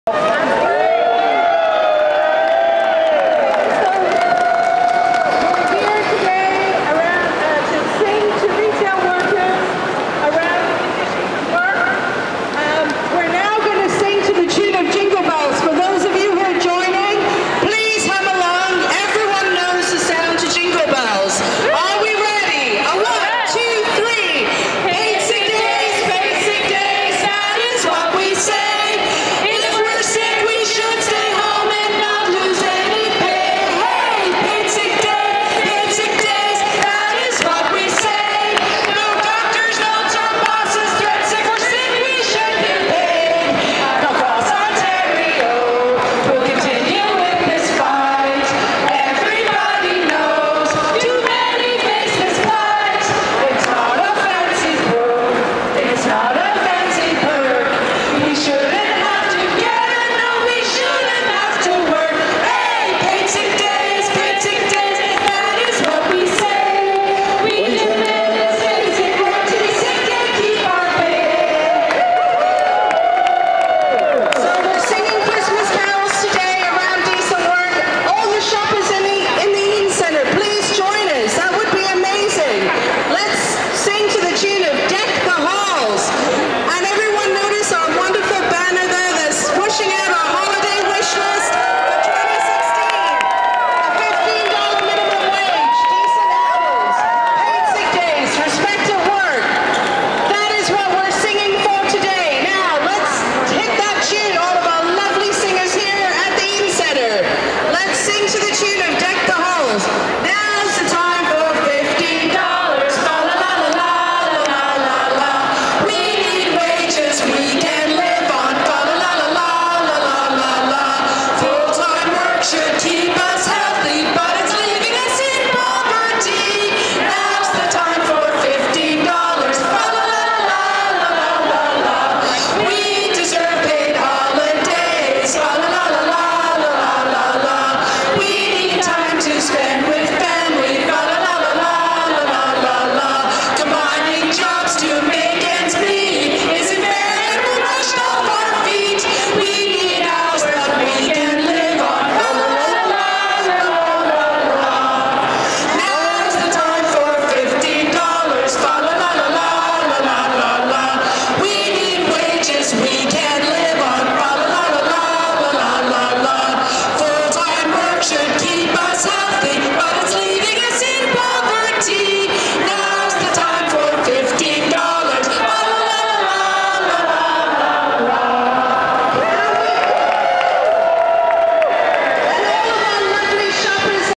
Holiday carolling for decent work
On December 12 the Fight for $15 and Fairness brought holiday cheer inside the Eaton's Centre, to celebrate the retail workers helping us prepare for the holiday season, and call on Premier Kathleen Wynne to make her new years resolution to support $15 minimum wage and decent working conditions